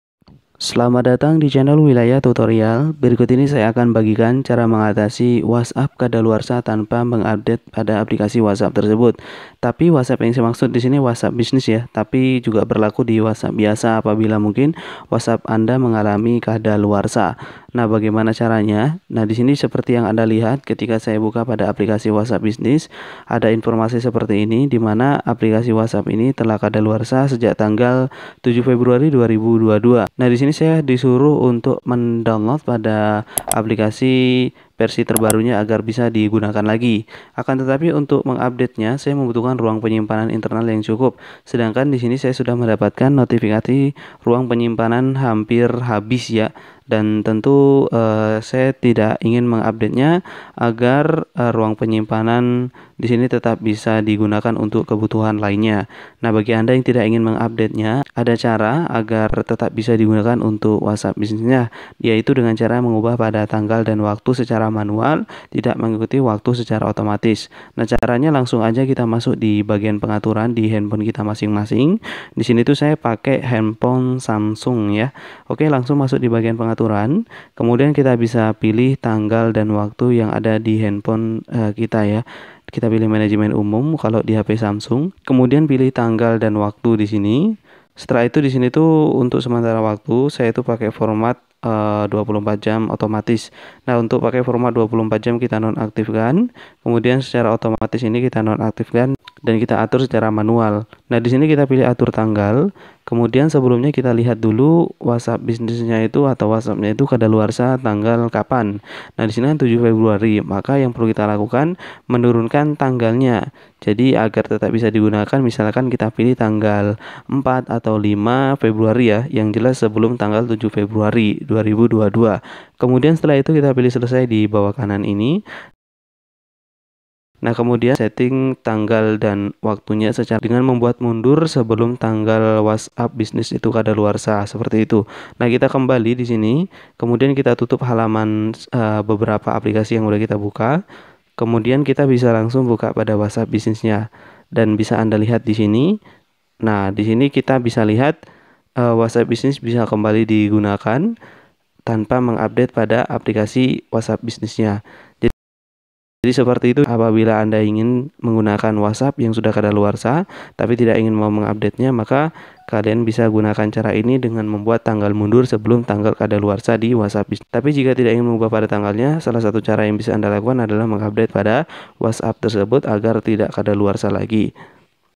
Cara Mengatasi WhatsApp Business Kadaluarsa Tanpa Update Aplikasi
Ketika mendapat notifikasi whatsapp business kadaluarsa maka salah satu yang perlu anda lakukan adalah uupdate aplikasi whatsappnya namun di video kali ini saya bagikan cara mengatasi whatsapp business kadaluarsa tanpa mengupdate aplikasinya.